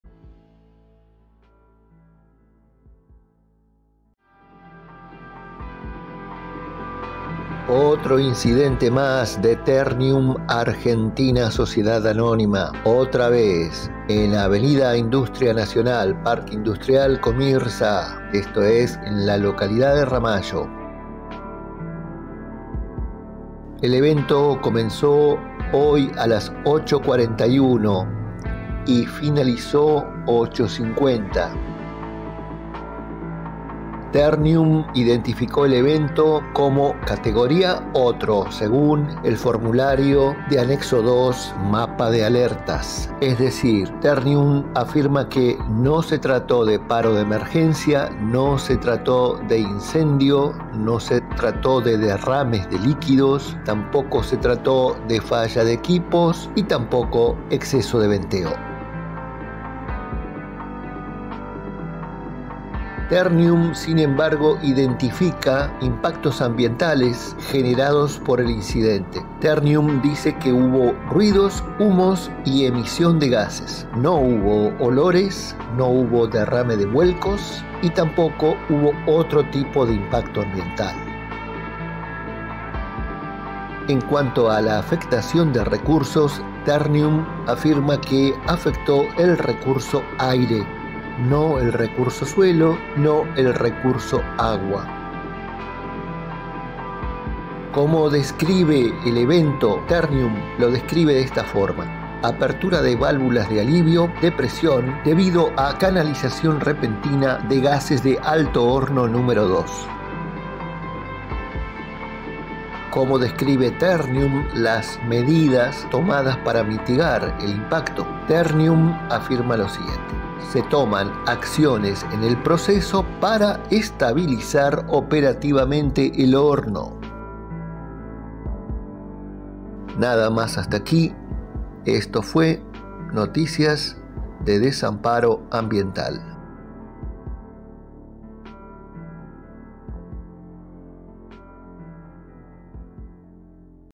TERNIUM ARGENTINA SA 17 05 2024 Mapa de Alertas
TERNIUM ARGENTINA SA apareció el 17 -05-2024 en el Mapa de Alertas del Ministerio de Ambiente de la Provincia de Buenos Aires. Fue un incidente en planta Ramallo. La compañía se limitó a identificar el evento como "Otro". Pero ruidos, humos y emisión de gases fueron los impactos ambientales generados.

El video se basa en la información que la empresa hizo pública en el formulario del Mapa de Alertas.
Si Ud. sabe, fehacientemente, que las cosas no ocurrieron del modo que informó la empresa, escríbanos.

Para acceder al Mapa de Alertas: visite la web del ministerio de Ambiente de la Provincia de Buenos Aires. En el Menú, clic en MAPAS y seleccione MAPA DE ALERTAS.